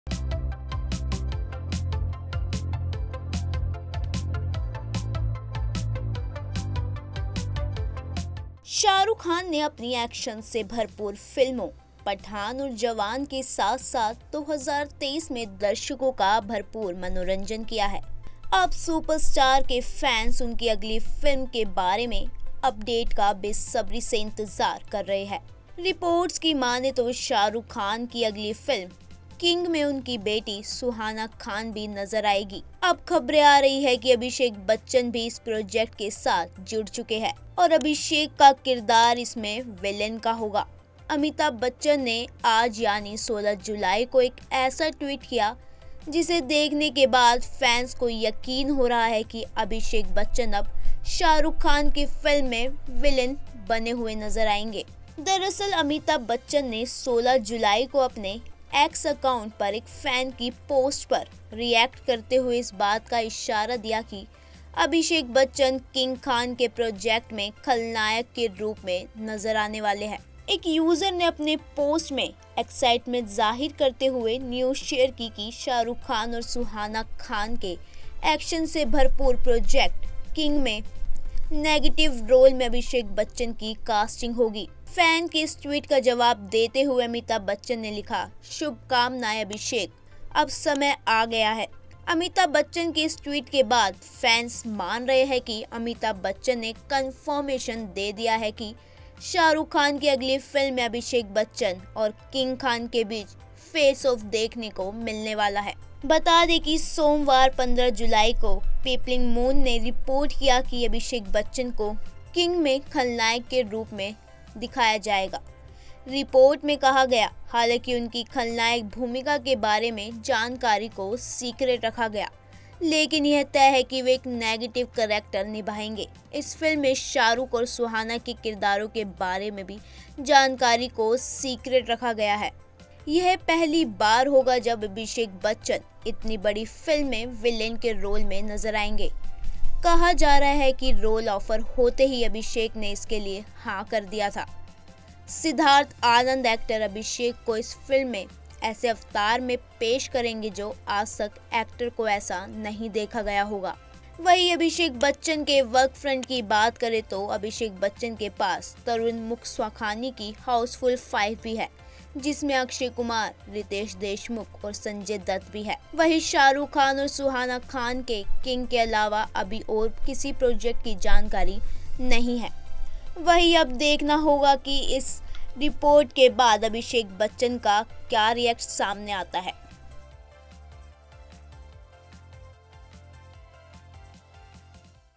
Abhishek Bachchan का नेगेटिव रोल में Shah Rukh से होगा सामना, किंग फिल्म पर अमिताभ का खुलासा
शाहरुख खान ने अपनी एक्शन से भरपूर फिल्मों 'पठान' और 'जवान' के साथ-साथ 2023 में दर्शकों का भरपूर मनोरंजन किया है. अब सुपरस्टार के फैन्स उनकी अगली फिल्म के बारे में अपडेट का बेसब्री से इंतजार कर रहे हैं. रिपोर्ट्स की मानें तो शाहरुख खान की अगली फिल्म 'किंग' में उनकी बेटी सुहाना खान भी होने वाली हैं